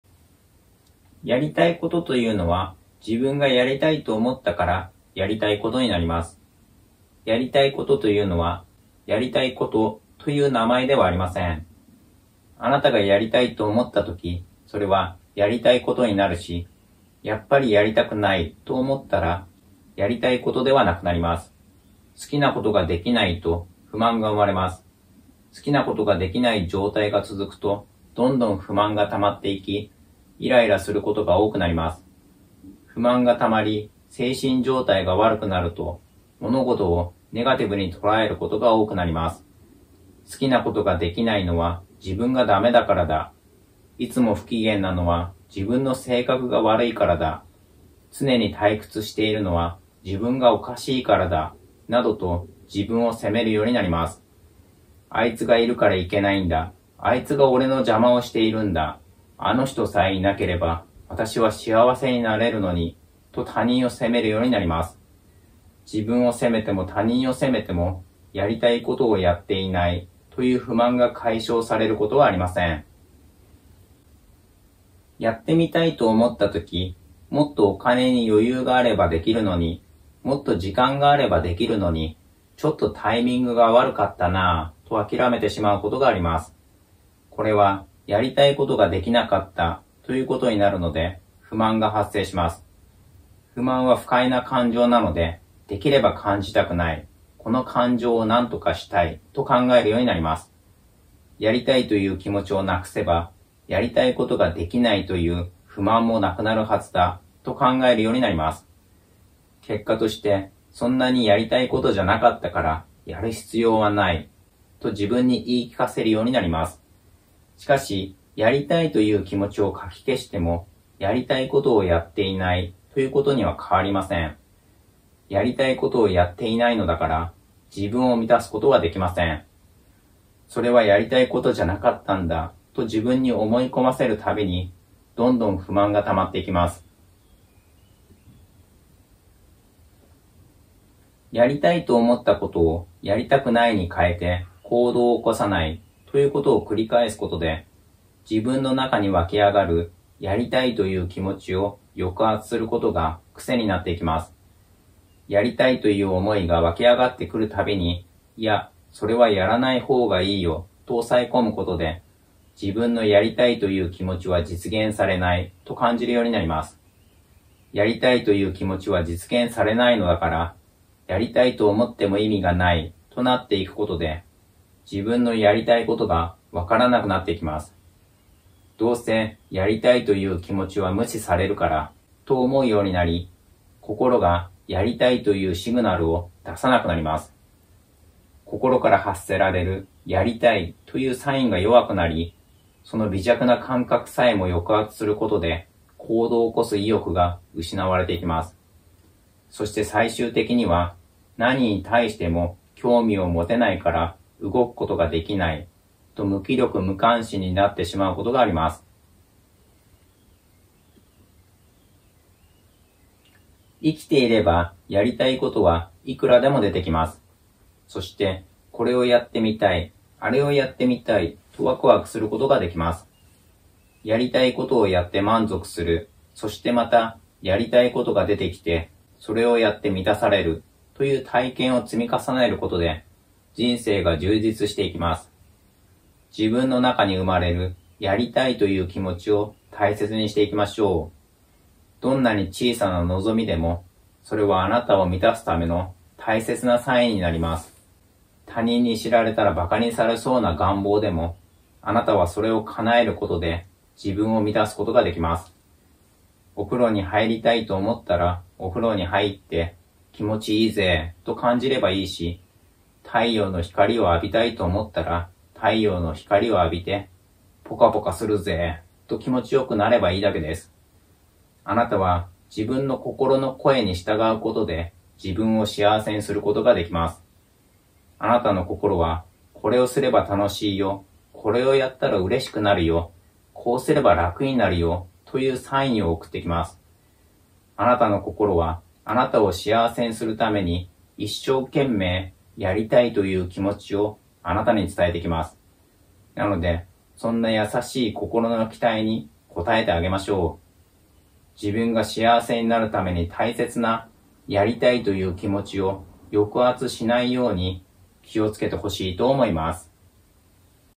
【字幕付き】「やりたい」という気持ちを「やらないほうがいい」に変えるから、やりたいことができなくて不満になる
▼えばっちのことまるわかりHP

▼えばっちの欲しいものリスト

【えばっちの他のチャンネル】

Visuals:  Pexels / Pixabay / ぱくたそ / いらすとや / OKUMONO
Video Editing : のりチロ